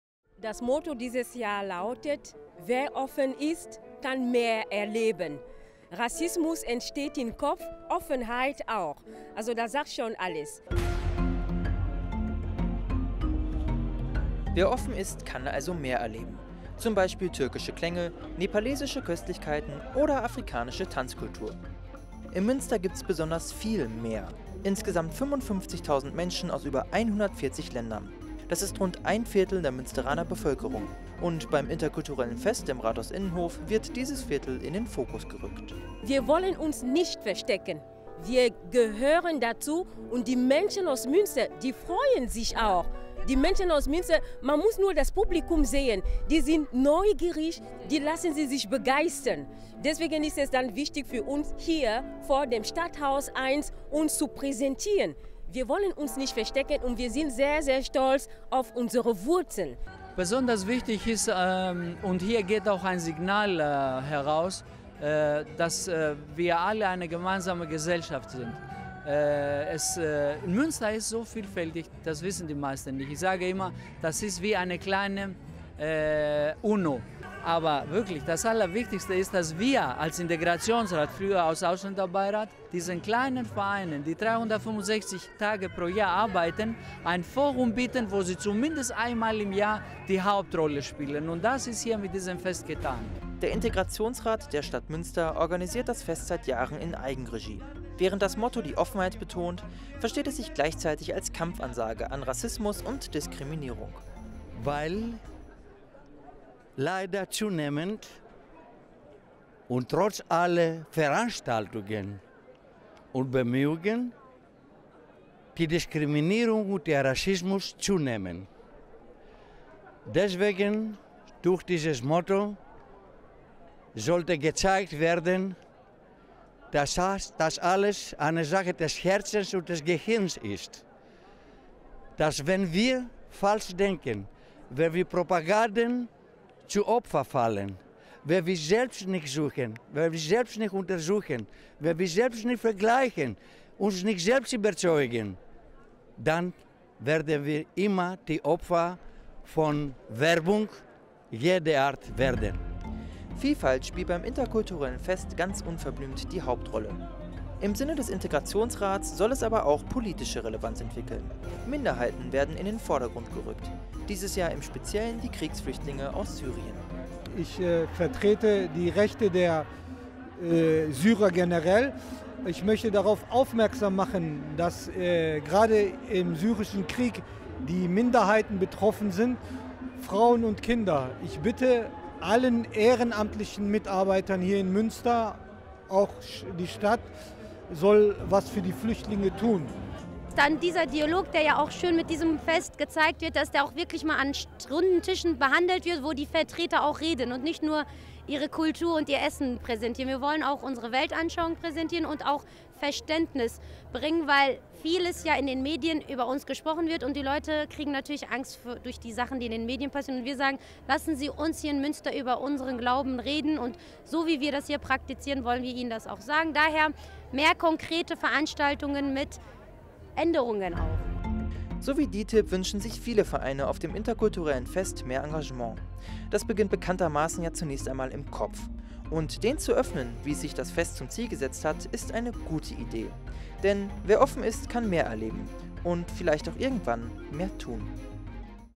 Das Interkulturelle Fest in Münster 2013
Ein Bericht vom Interkulturellen Fest in Münster, aufgenommen am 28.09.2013 im Rahmen der Interkulturellen Woche.